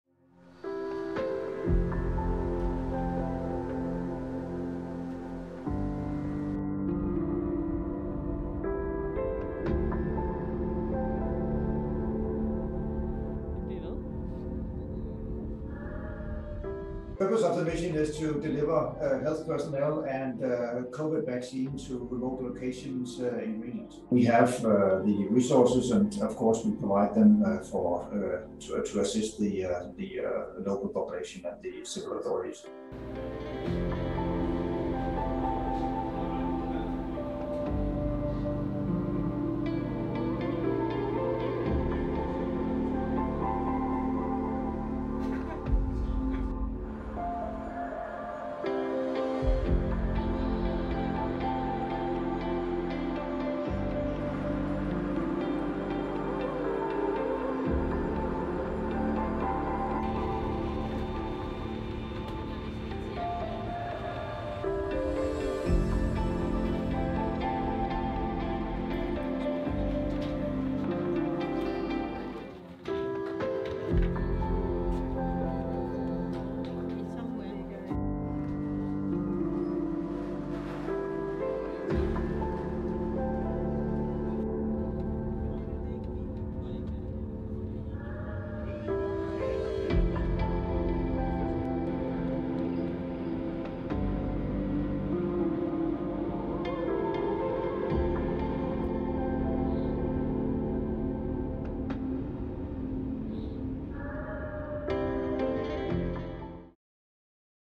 Au Groenland, la marine danoise livre les vaccins dans les villages isolés | AFP
Pas question que la dispersion de l'habitat freine la vaccination anti-Covid: l'armée danoise a commencé la livraison de vaccins dans des villages côtiers isolés au Groenland, un exercice compliqué par le froid extrême.